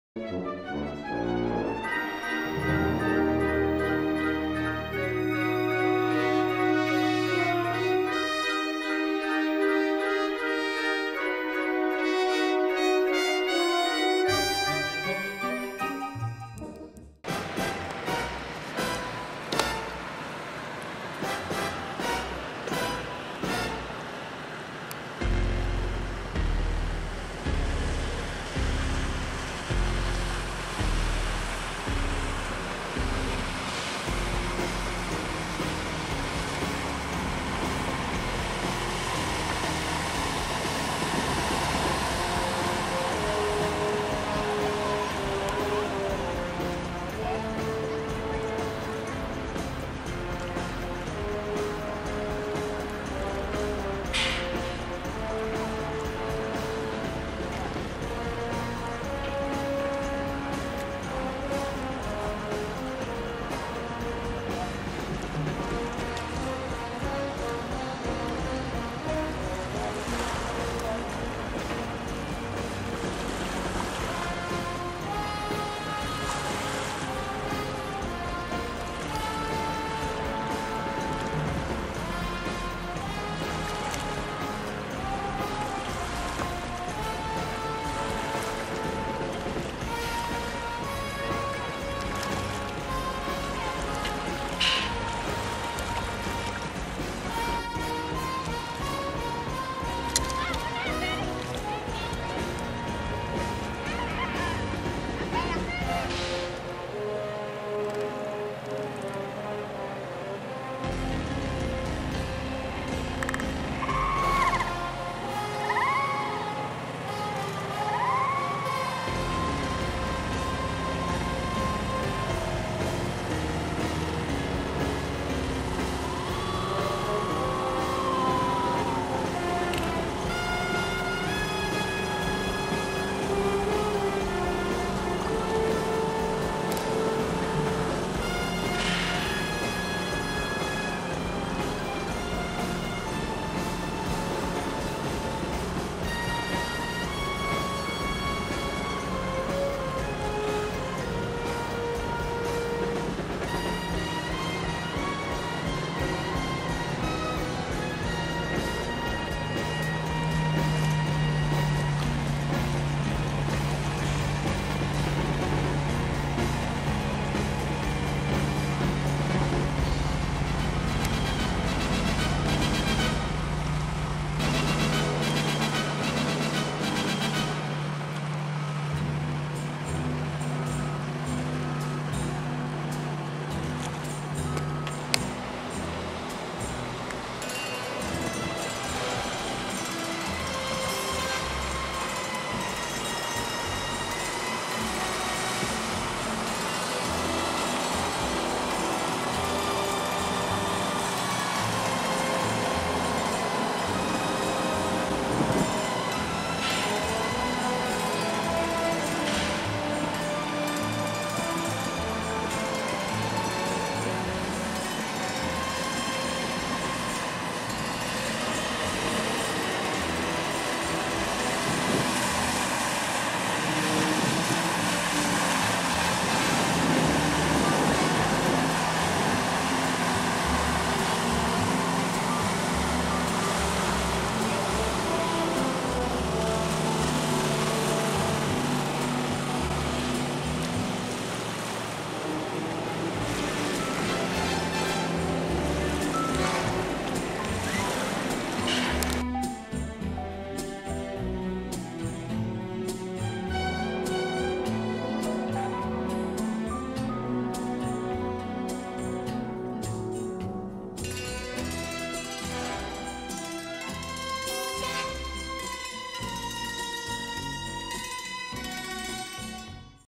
Wake Board Kids
This vid shows two kids wake boarding as their father powers the boat in  Florida.